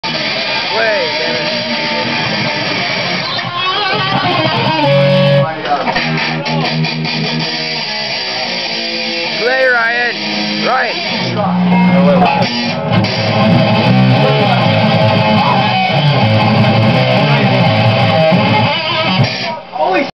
Battle!
Nick and Ryan are at it on guitar. sorta...

We wanted them to battle, but it did not happen really...